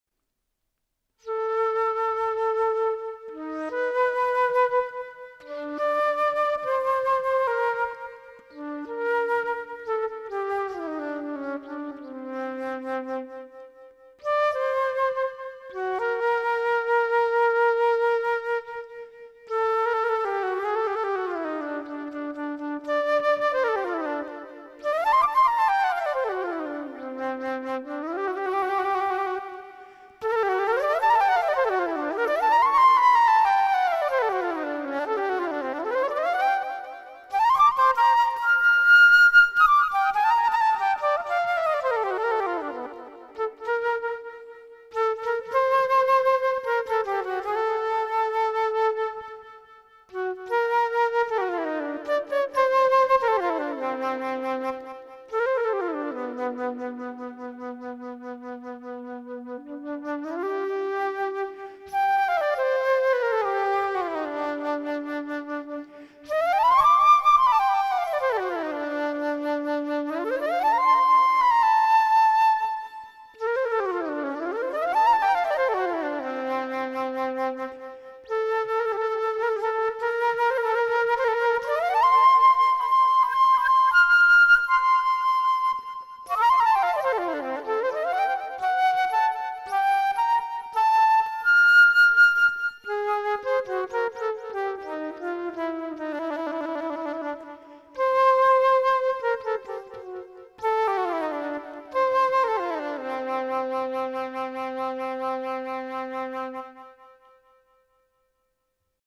Birdsong Flute: Joyful Girls with Pastel-Colored Hair
Joyful girls with pastel-colored hair are accompanied by adorable, large-eyed birds in a vibrant setting. The girls smile warmly as they interact with the colorful, whimsical creatures that seem to be their companions.